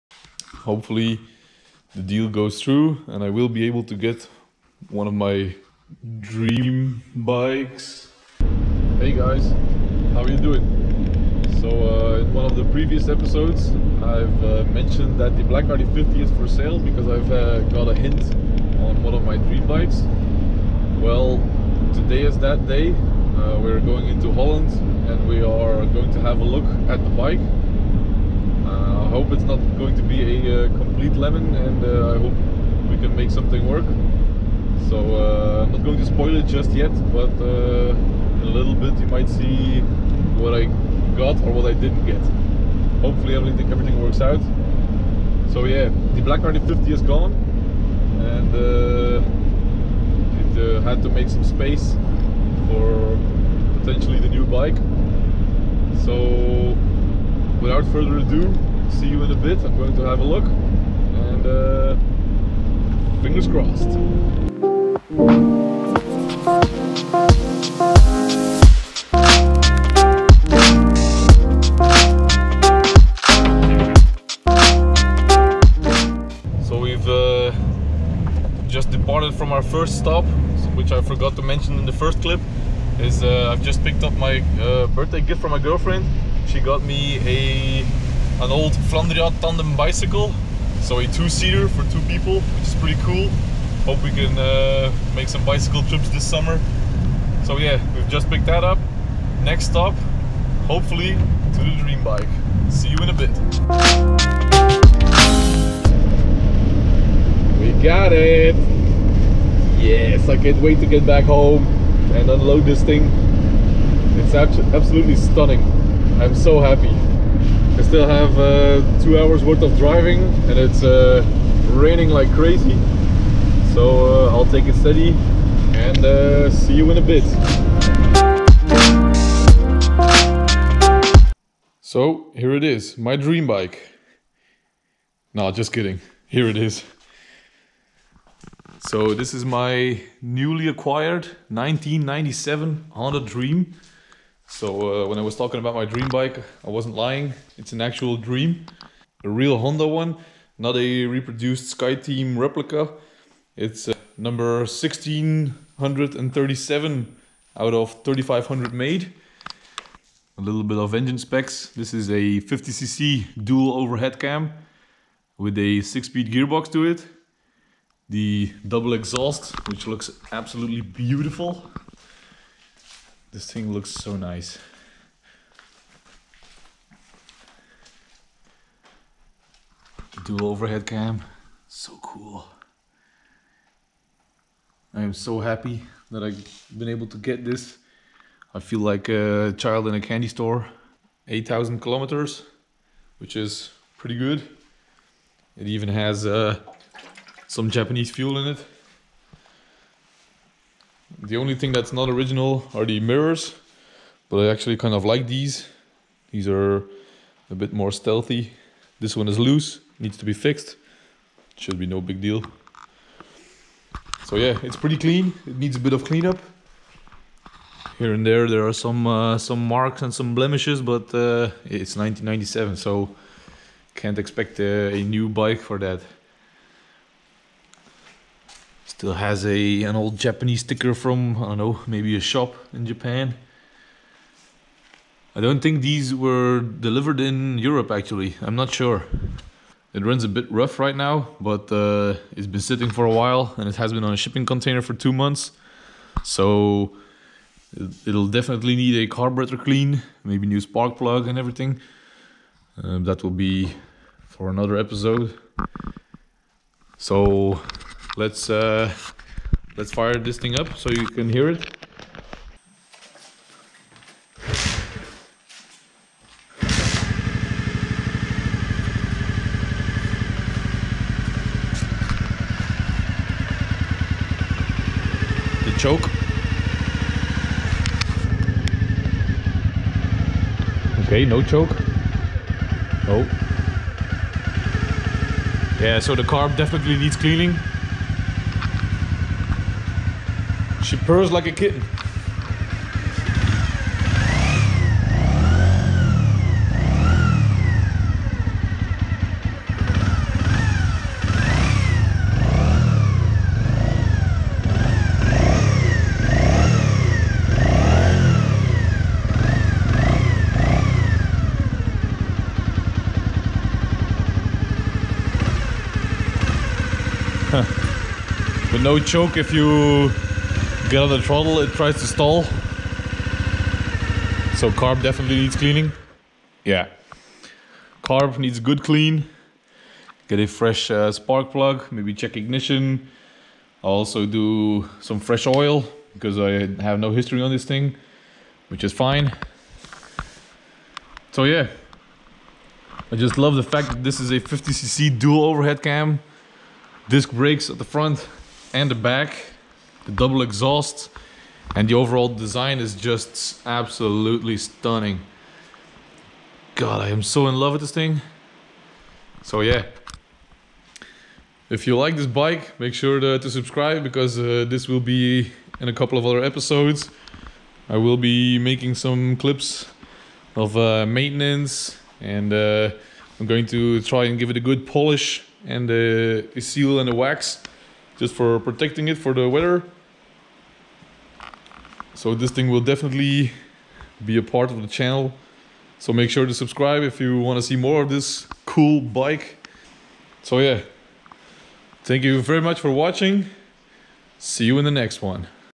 Getting my dream bike!
In this episode I'm taking a trip to Holland to go and have a look at my Dream bike!

1997 Honda Dream 50
Honda AC15